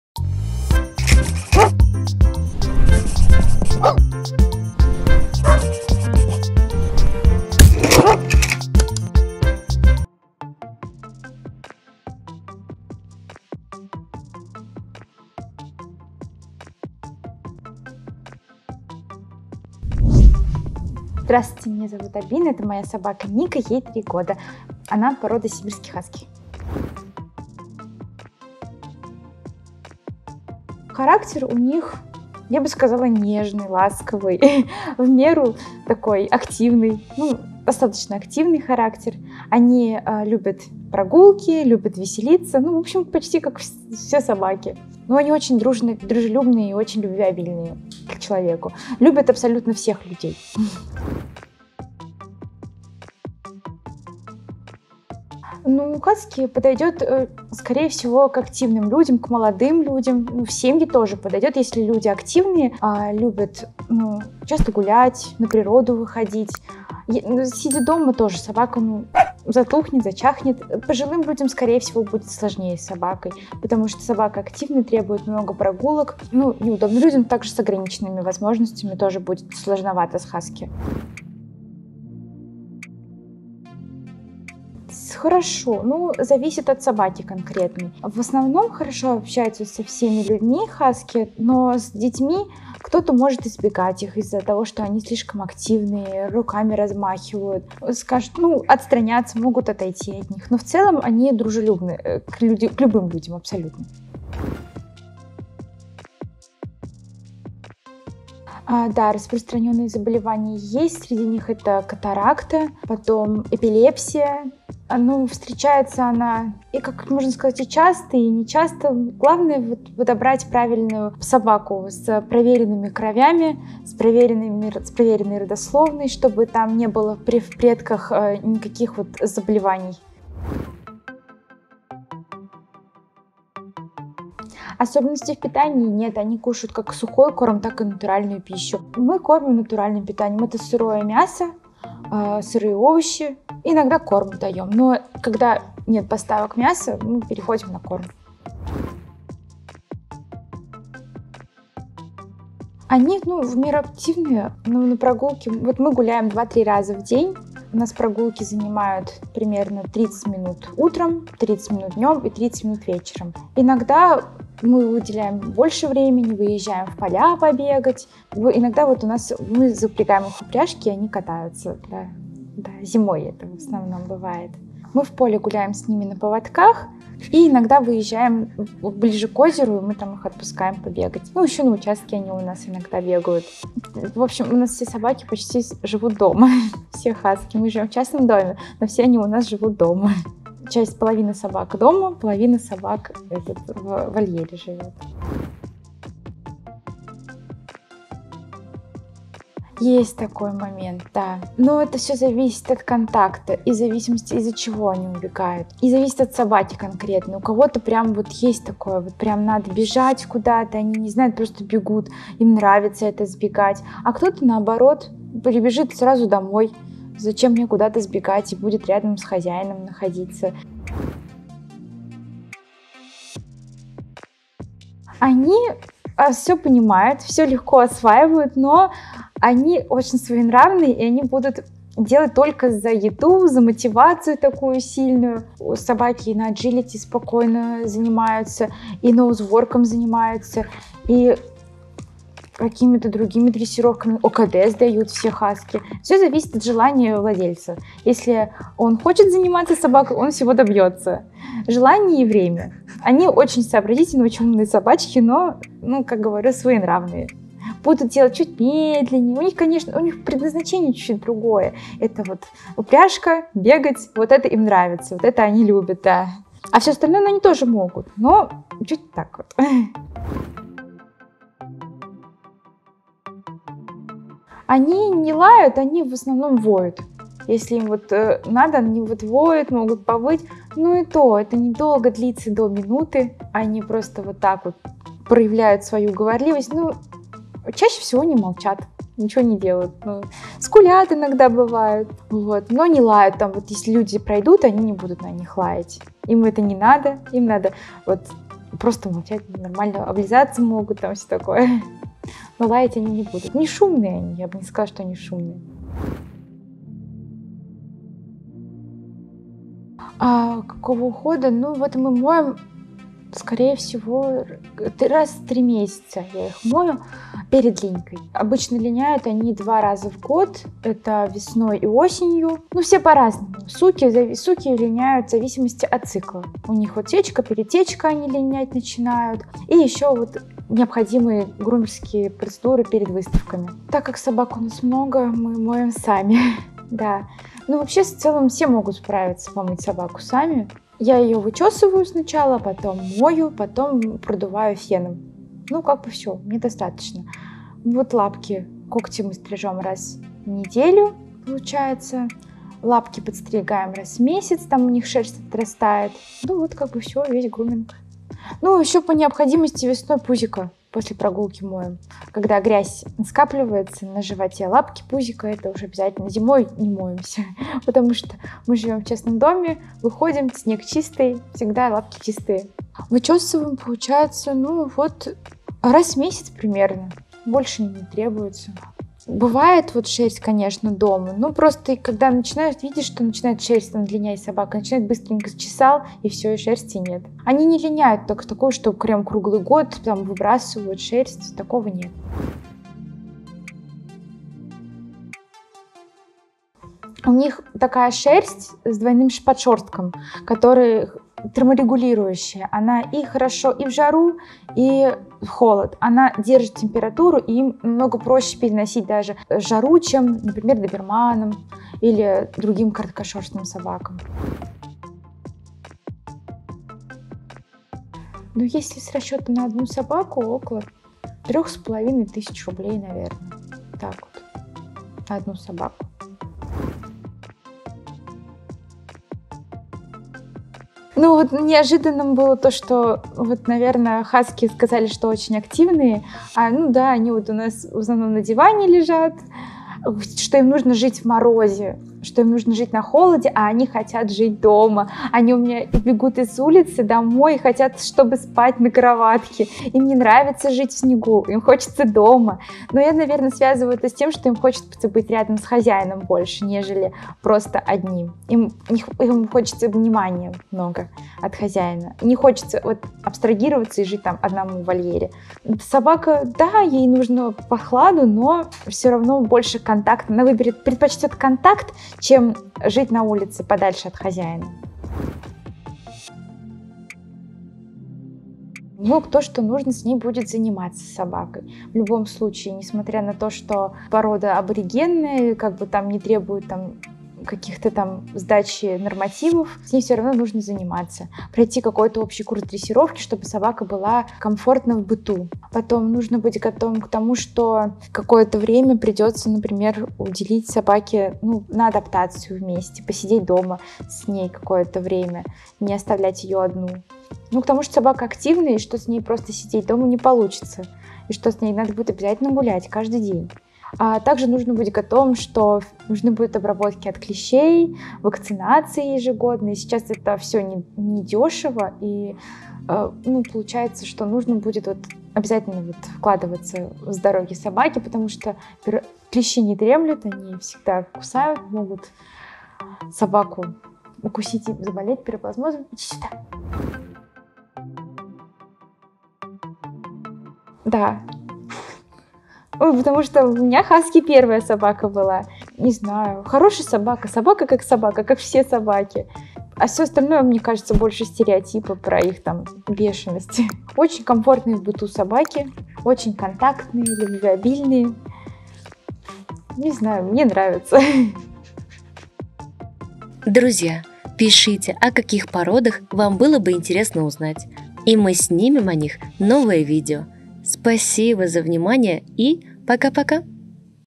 Сибирский хаски: все о породе, плюсы и минусы — шоу «Собака: ожидание/реальность»
Если вы ищете породу, которая была бы на позитиве 24/7 и любила вас так, как никто на свете, - вы ее нашли!

В гостях нашей рубрики "Ожидание/реальность" Альбина и ее сибирский хаски Ника. Комфортная в быту, контактная, любвеобильная - как хозяйка характеризует свою улыбчивую красотку. Хаски очень любят гулять, веселиться, играть и контактировать с людьми и сородичами 🐕‍🦺

Также в нашем новом видео мы развенчиваем мифы! Правда ли, что хаски часто сбегают? Или что они постоянно воют? И верно ли, что из-за густой шерсти им тяжело в жару?

Рекомендуем домашний уход для данной породы от Agree’s for pets:

✔️Шампунь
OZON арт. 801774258
✔️Спрей-кондиционер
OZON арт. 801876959
✔️Мыло для лап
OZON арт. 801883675
✔️Воск для лап

Эпизоды:
00:00 Поехали!
00:10 Кратко о хаски
00:20 Знакомство с героями Альбиной и Никой
00:28 Какой характер у хаски?
00:52 Каким людям, в какую семью подойдет хаски?
01:23 Как хаски взаимодействуют с чужими людьми, детьми?
01:46 Есть ли у хаски распространенные проблемы со здоровьем?
02:12 Есть ли особенности в питании? Прихотливы ли хаски к еде?
02:30 Насколько активны хаски? Как часто и долго вы гуляете со своей собакой?
03:16 про хаски часто говорят, что они склонны к побегам, так ли это?
03:42 Как быстро обучаются хаски? Легло ли осваивают команды?
04:31 Насколько «говорливы» хаски? Можно ли назвать породу шумной?
05:07 Какого ухода требует хаски? Как часто моете?
06:33 Правда ли, что хаски тяжело переносят жару из-за густой шерсти?
06:56 Сколько примерно в месяц вы тратите на свою собаку?
07:08 Что было самым неожиданным в воспитании вашей собаки?
07:57 К чему в первую очередь нужно быть готовым человеку, который хочет завести хаски?
08:57 Рекомендовали бы вы хаски в качестве первой собаки?